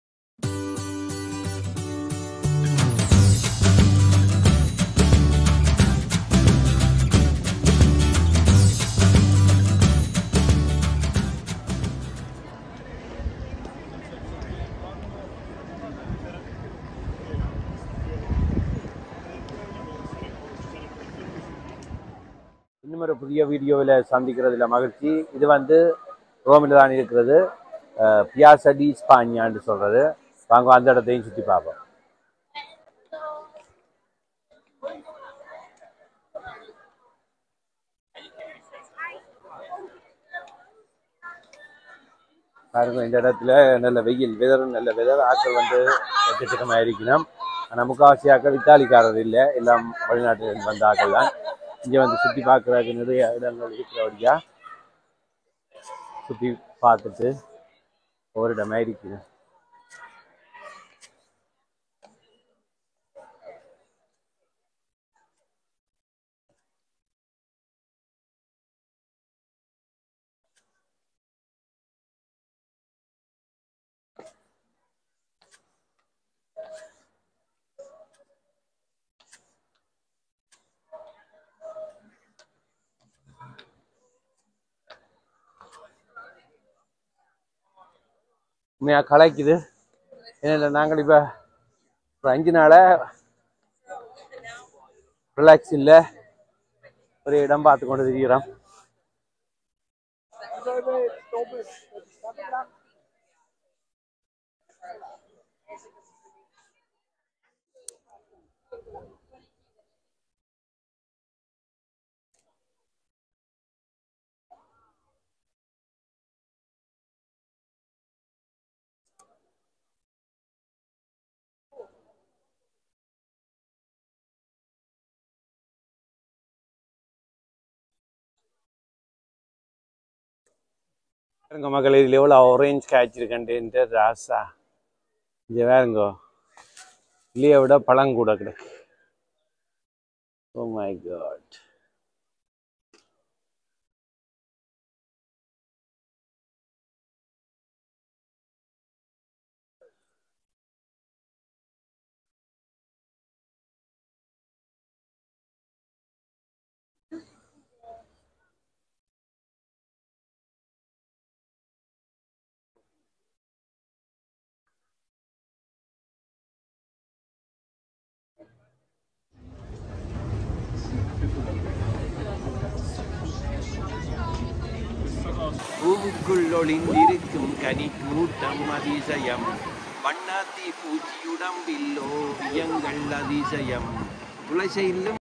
Piazza di Spagna | One of the most famous squares in Rome, Italy 🇮🇹 | 2023
Piazza di Spagna ("Spanish Square"), at the bottom of the Spanish Steps, is one of the most famous squares in Rome, Italy. It owes its name to the Palazzo di Spagna, the seat of the Embassy of Spain to the Holy See. There is also the famed Column of the Immaculate Conception of the Blessed Virgin Mary.